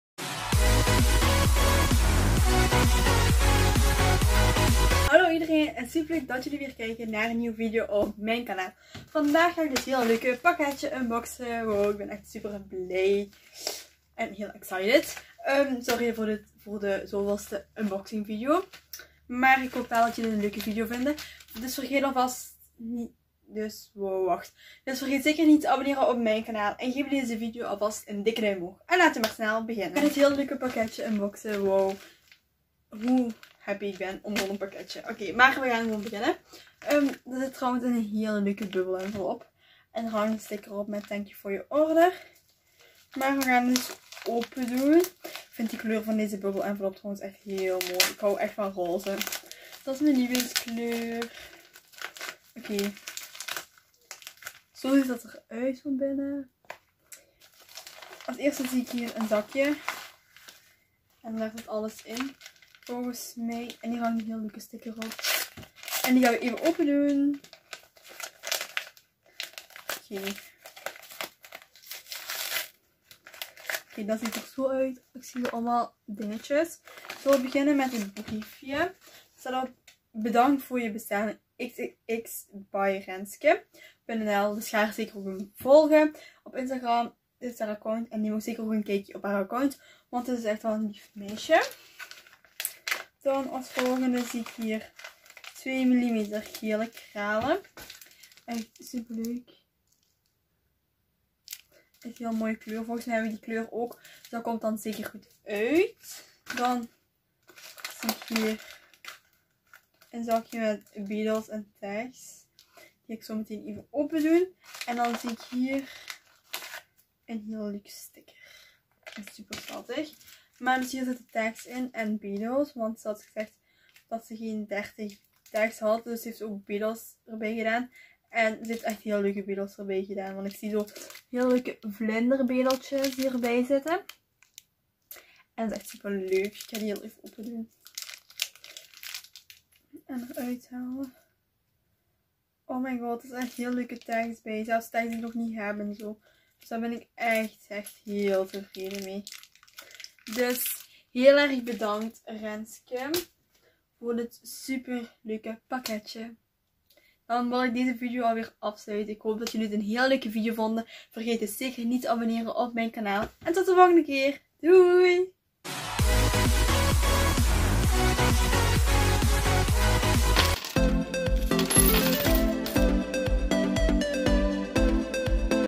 UNBOXING VIDEO💖 || Things By Noralie #25
In deze video ga ik een heel leuk pakketje unboxen, veel kijkplezier!